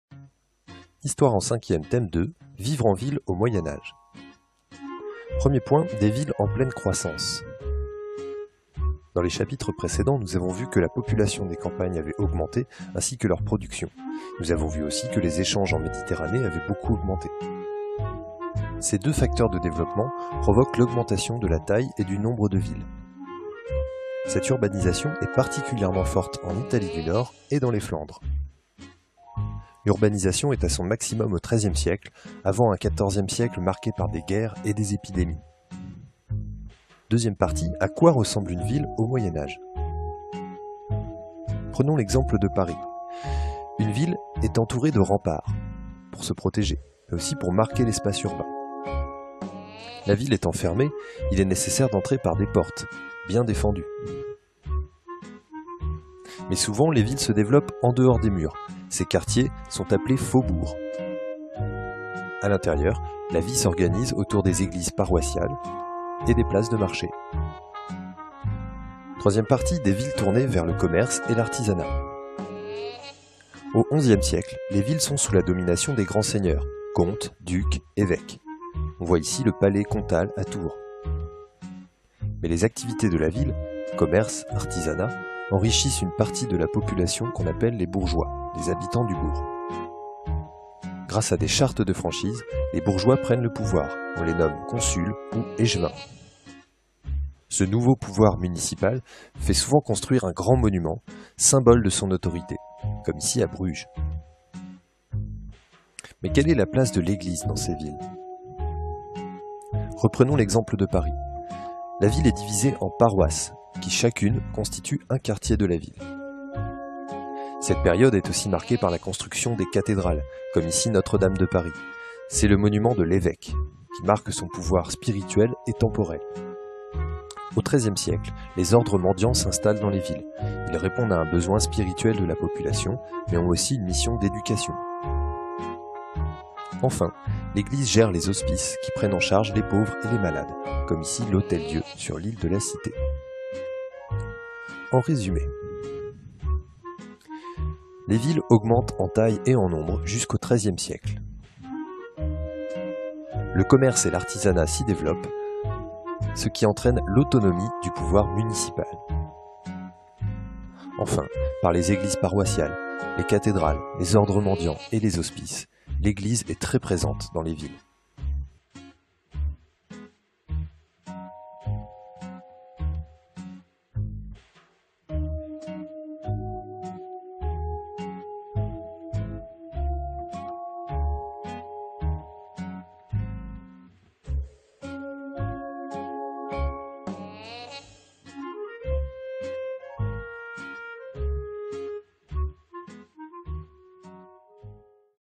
Vivre en ville au Moyen Âge
Chapitre d'histoire en cinquième : le développement des villes au Moyen âge.